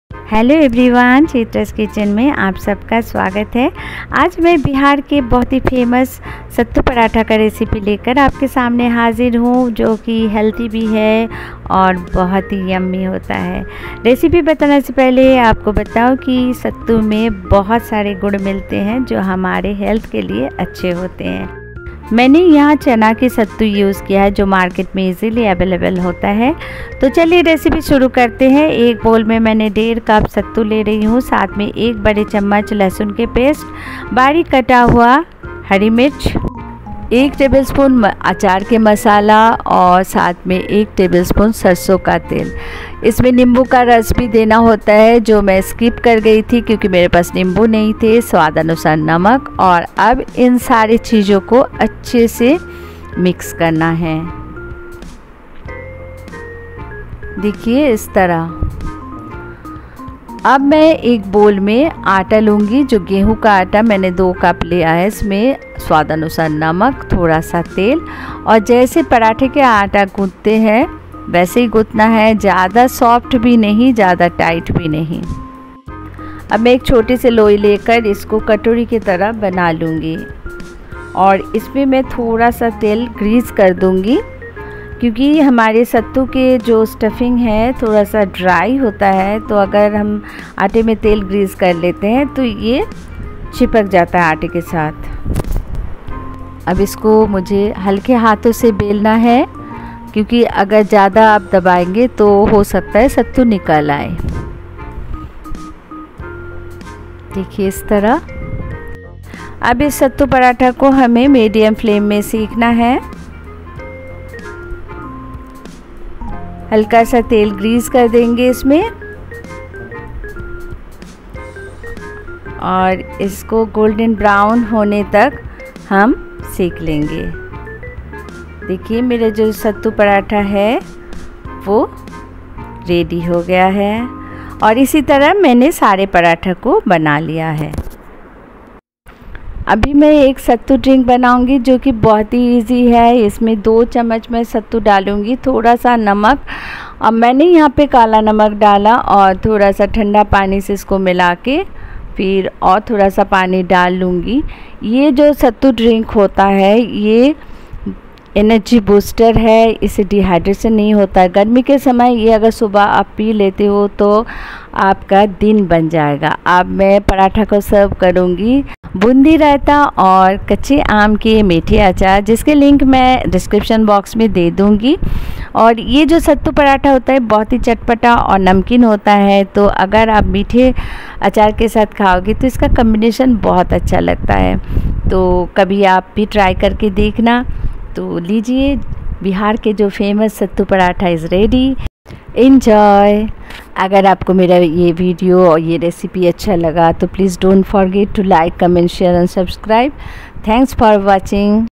SATTU PARATHA | बिहार के फेमस सत्तु पराठे | BREAKFAST RECIPE |REFRESHING SATTU DRINK | CHITRA SANYAL
SATTU PARATHA | बिहार के फेमस सत्तु पराठे | BREAKFAST RECIPE | EASY SATTU REFRESHING | CHITRA SANYAL